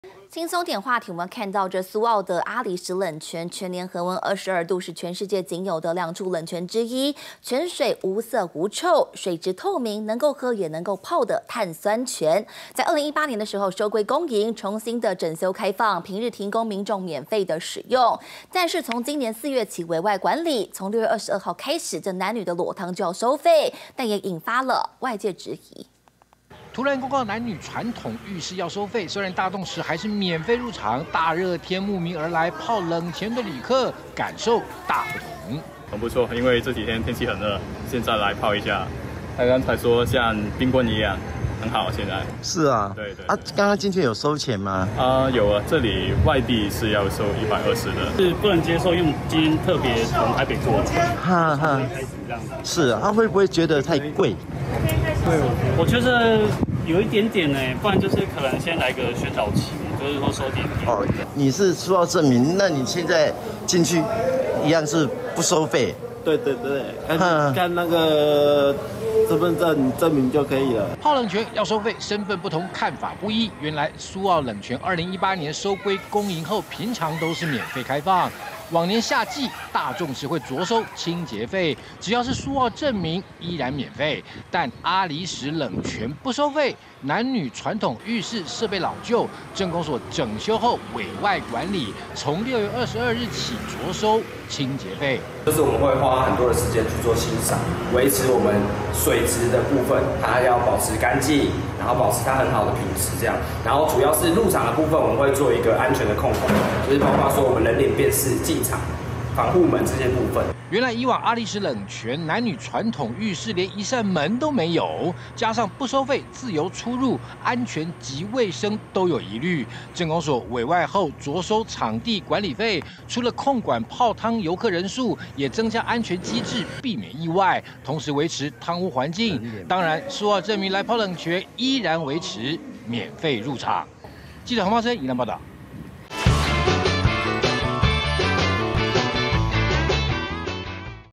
蘇澳「阿里史」冷泉22日起收費 「全票120元」引發爭議
位於蘇澳的阿里史冷泉全年恆溫22度，是全世界僅有的兩處冷泉之一，由於泉水無色無臭、水質透明，是能喝也能泡的碳酸泉。2018年收歸公營後重新整修開放，平日提供民眾免費使用，不過今年4月起冷泉委外管理，從6月22日起男女裸湯要開始收費，雖然大眾池依然免費，但也引發部分來訪的民眾質疑。